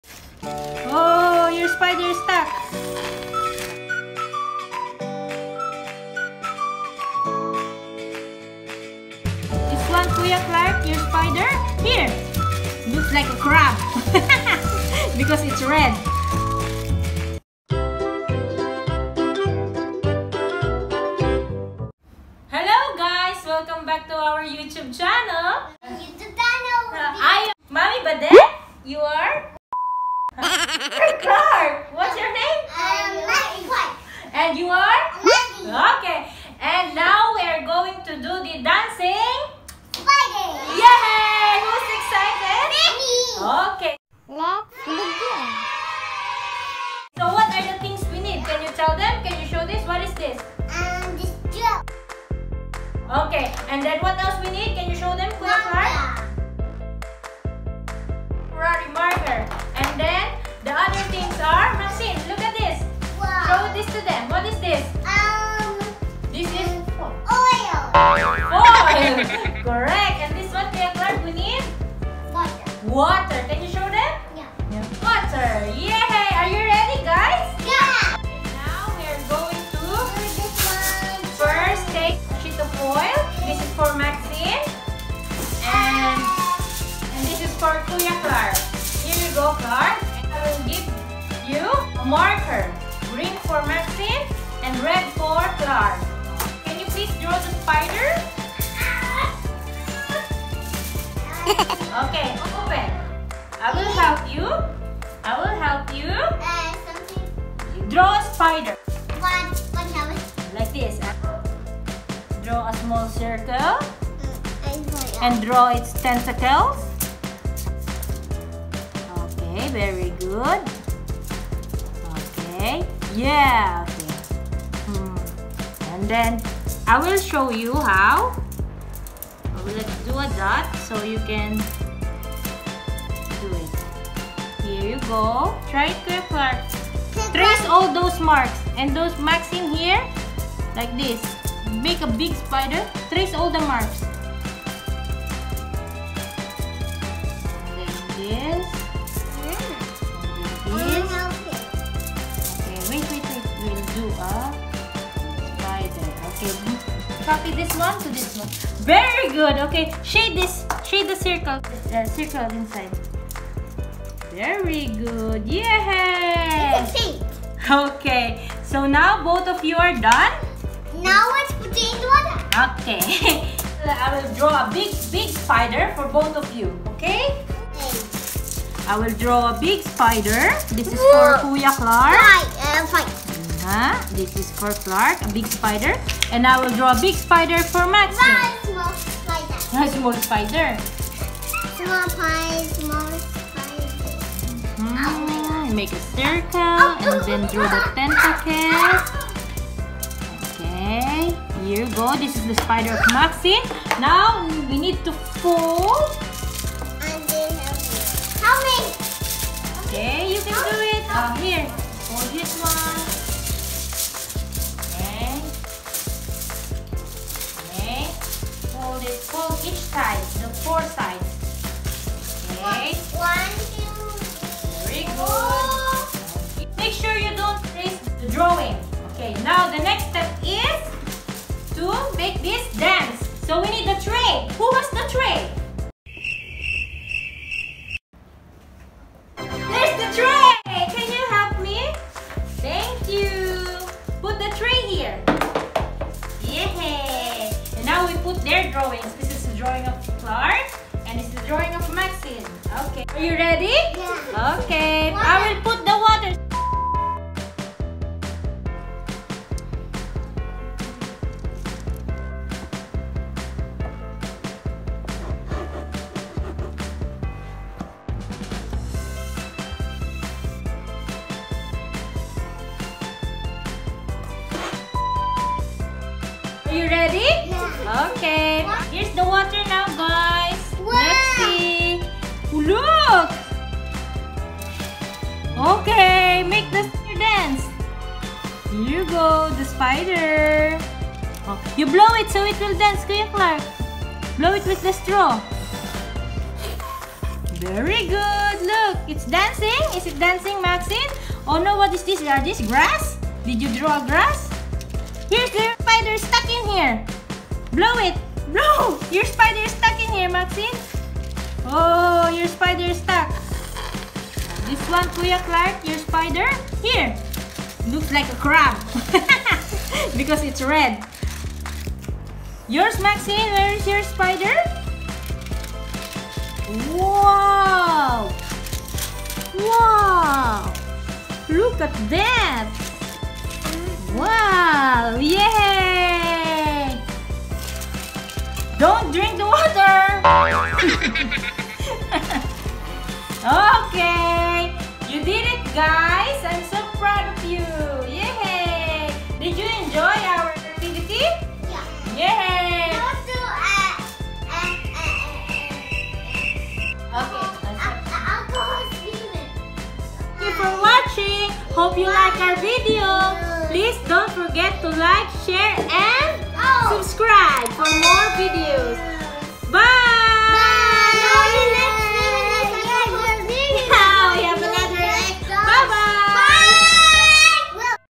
Kids Experiment (Dancing Spider)
Just using foil, temporary marker and water we made a kid experiment called “Dancing Spider”.